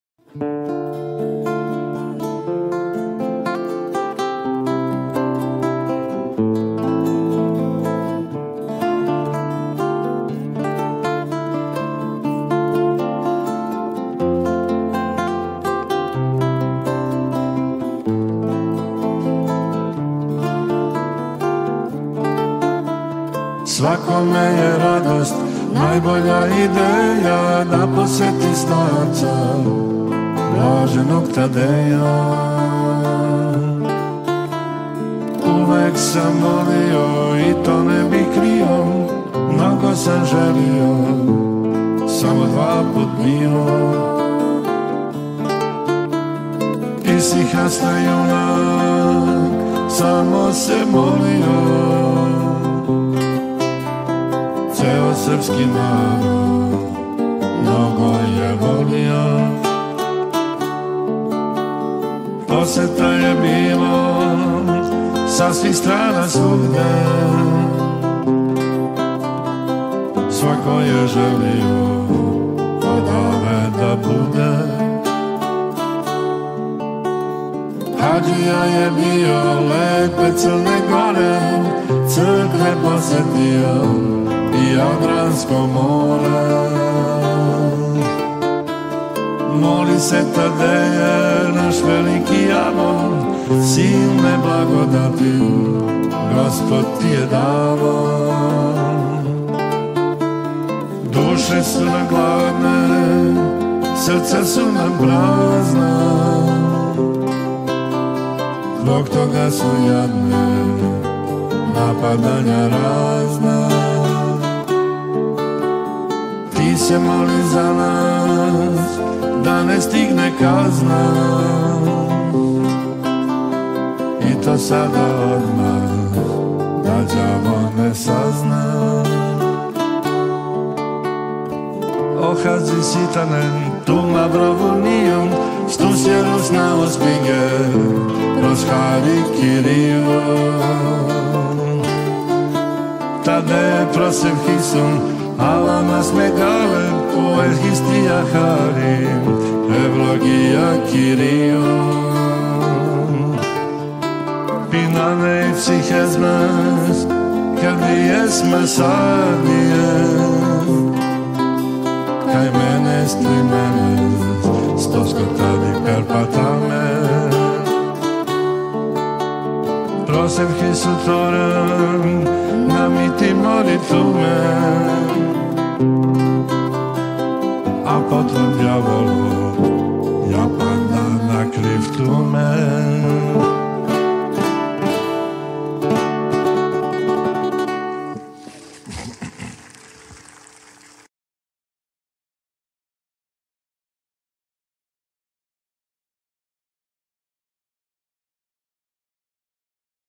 Хуманитарни концерт дечије духовне музике
...у организацији манастира Свете Тројице, из Бјелих Вода код Љубовије. биће одржан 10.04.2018. у дому културе Чукарица, Тургењева 5, са почетком у 19:00 часова.